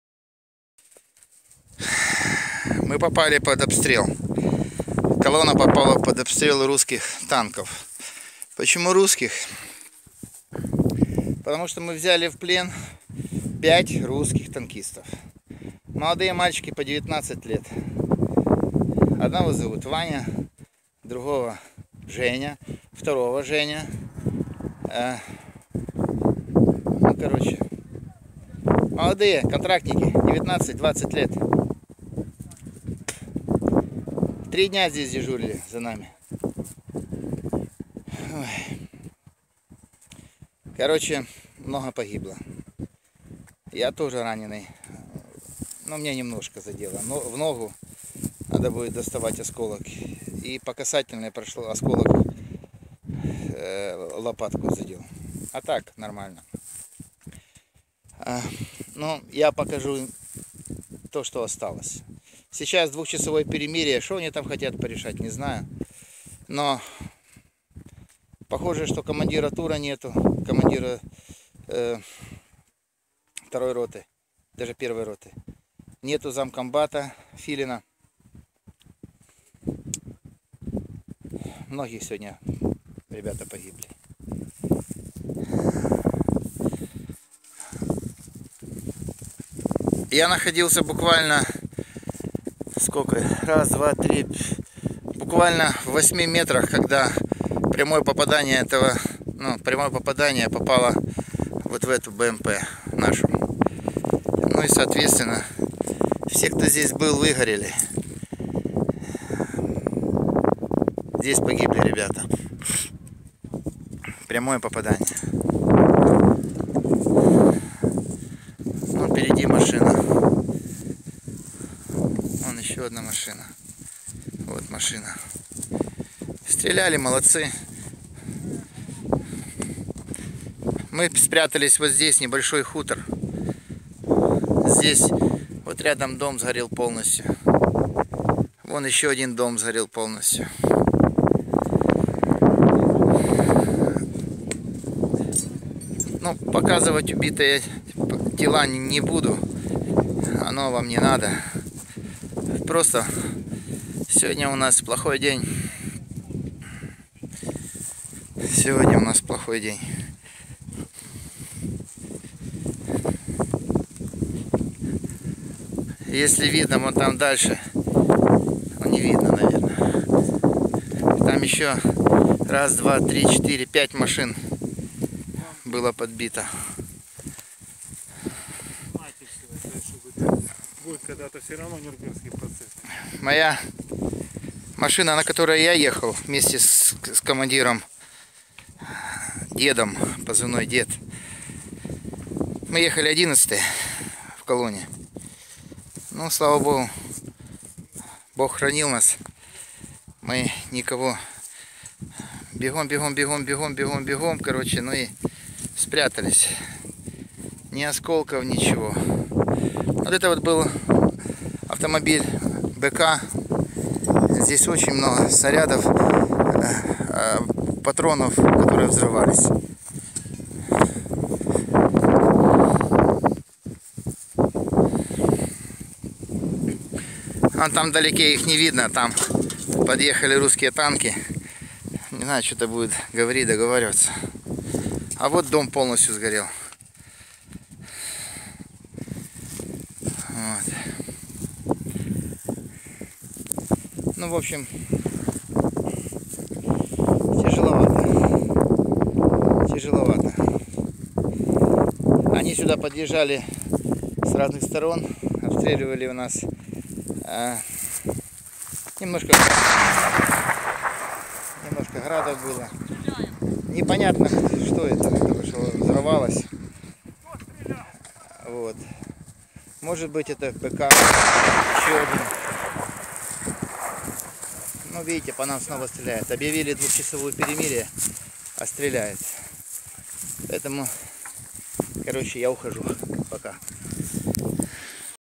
Мы взяли в плен 5 русских танкистов - украинский боец
Это доказательства участия российской регулярной армии (конкретно российской военной техники Т-72Б3) в убийстве мирных жителей и украинских военных на территории независимого государства Украина. Часть 1.
Подбитые российские танки Т-72Б3 в августе 2014 снял на свой телефон мой знакомый - позывной "Пастор" из бт «Донбасс». Вместе с колонной бт «Донбасс» боец выходил из Иловайского окружения по тому "зеленому коридору", о котором договорились военачальники РФ и Украины для наших добровольцев. Пообещав «зеленый коридор», русские заранее окопались и подготовились, поставив свои танки по всей длине "зеленого коридора" (цена слова русского офицера). А затем хладнокровно, как в тире, русские расстреливали украинских добровольцев из танков. Но «донбассовцы» дали бой (не единственные). Отсюда и сгоревшие русские танки, и взятые в плен несколько россиян. Пастор рассказывает детали боя и о русских, "которых там не было" .